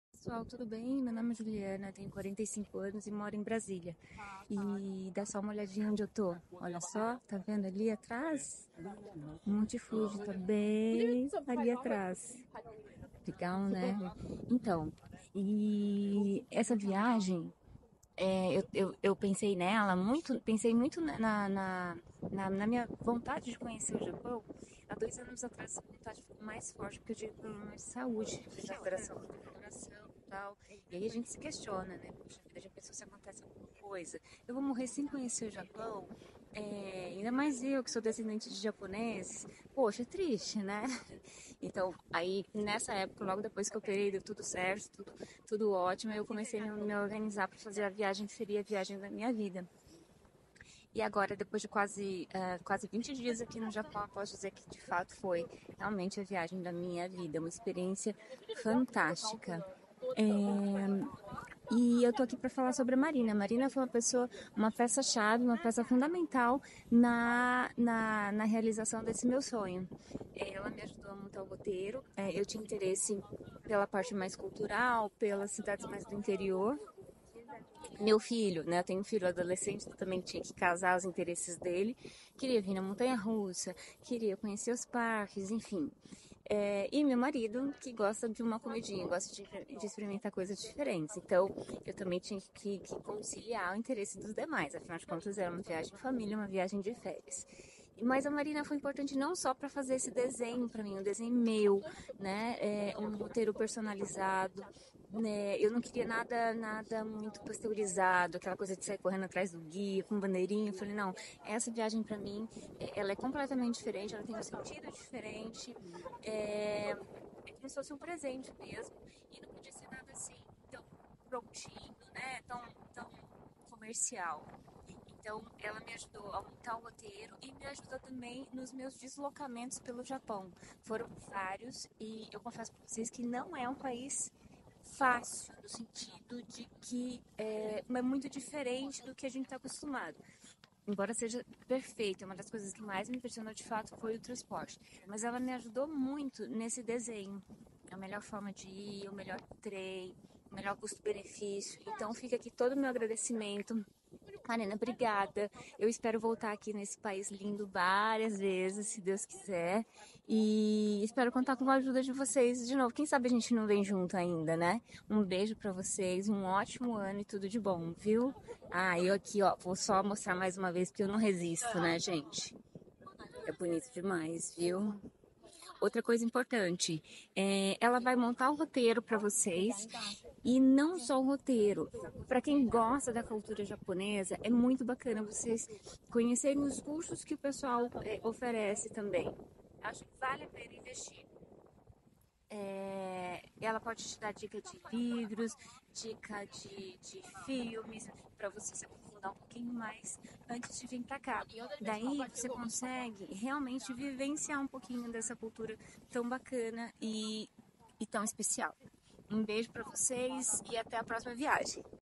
Viagem que era um Sonho de Vida, virou realidade | Clientes de Roteiro Personalizado Ultradetalhado
A Ju Nonaka, sempre sonhou em conhecer o Japão. Além do apreço cultural ela tem raízes japonesas. Após um problema de saúde no coração, decidiu que era a hora de tirar esse sonho do papel.

Realizou esse sonho em família, conciliando seu gosto pela cultura tradicional, com o do filho por parques e aventura e o do marido com foco na gastronomia.

Eles são clientes do Roteiro Personalizado Ultradetalhado, a solução ideal para quem quer tudo pronto, sem investir muito tempo no planejamento.